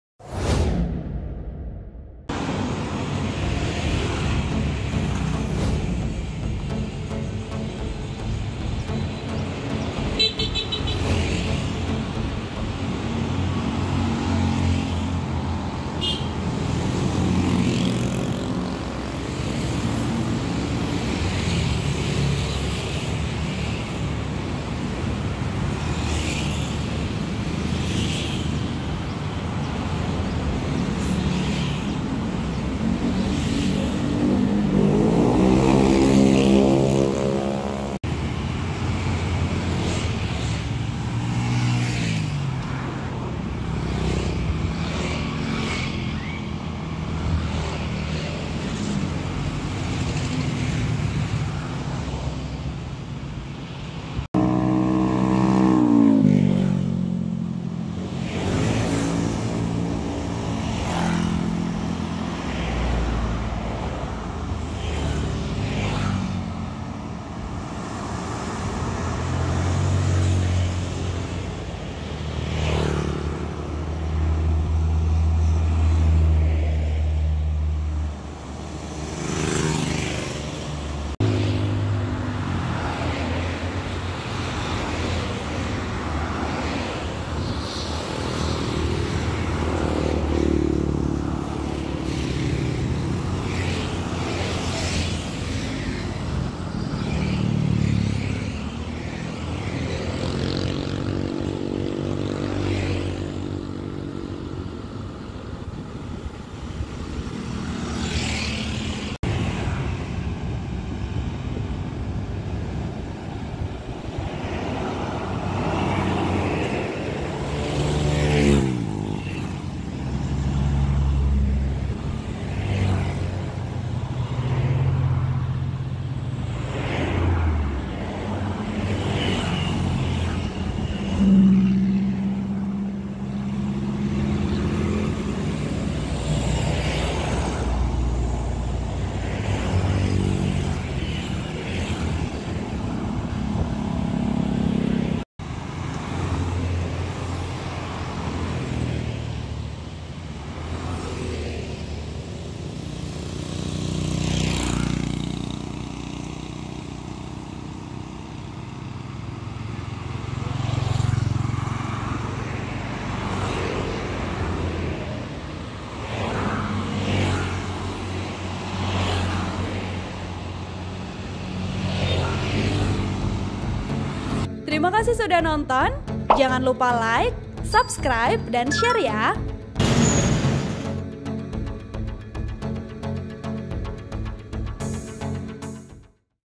KONDISI LALU LINTAS ARUS BALIK DI JALAN ARTERI CIAMIS-BANJAR TERPANTAU RAMAI LANCAR
Laporan: Ai Sani Nuraini | Video Editor: Edwin Tk